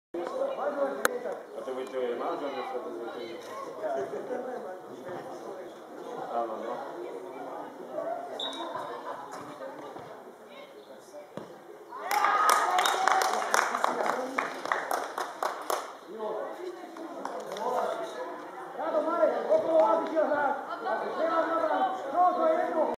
Adam Haspra 1:1 (Topoľčianky U15 - Nevidzany U15 1:7)
Súťaž žiakov ObFZ Nitra skupina B - 11.kolo, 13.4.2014 o 13:00, Topoľčianky

TOPOĽČIANKY U15 - NEVIDZANY U15 1:7 (1:1)